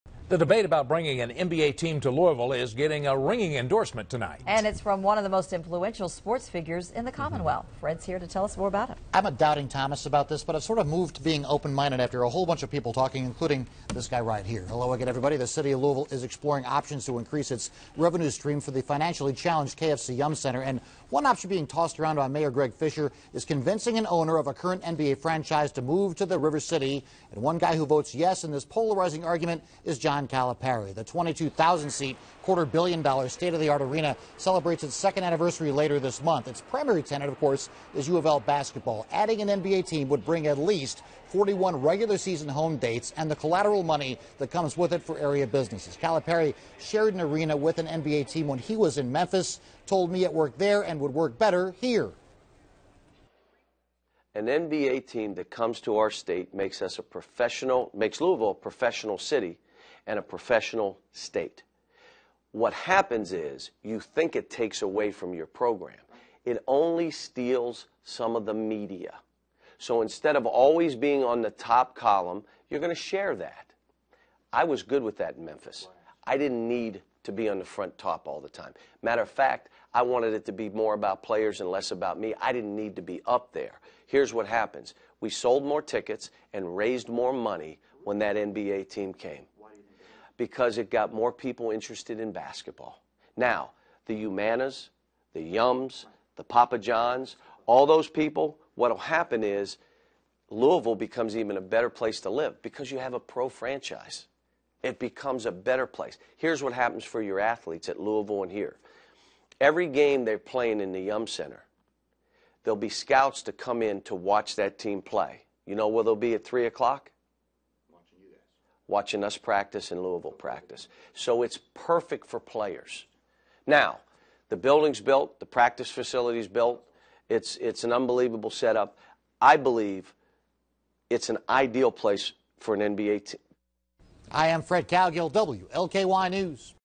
Coach Cal supports NBA in Louisville
University of Kentucky Head Basketball Caoch John Calipari supports the idea of an NBA team playing in Louisville at the KFC Yum Center.